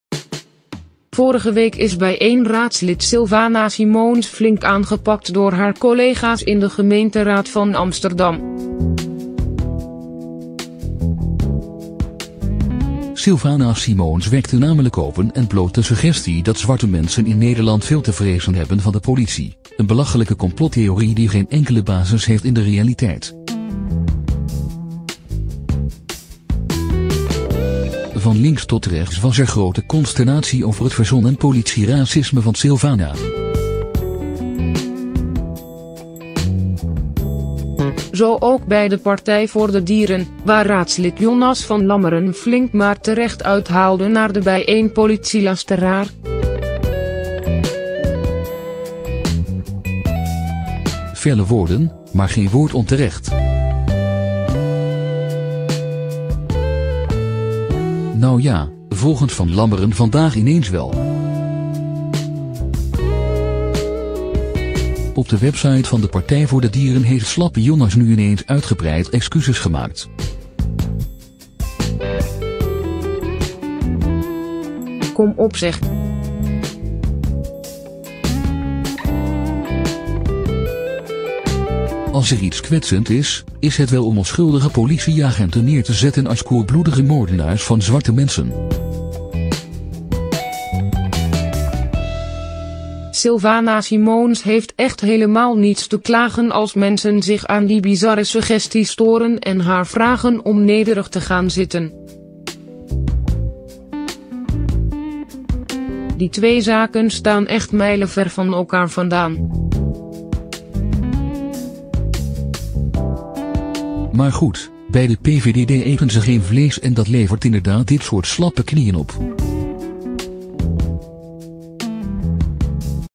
Slappe hap! PvdD-raadslid Johnas van Lammeren smeekt bij politiehater Sylvana op z’n knietjes om ver
Slappe hap! PvdD-raadslid Johnas van Lammeren smeekt bij politiehater Sylvana op z’n knietjes om vergiffenis

► Registreer voor meer informatie:
► Neem voor vragen over auteursrechten contact op met:

Afbeelding van de CC-licentie:
Https: // Plaughter.com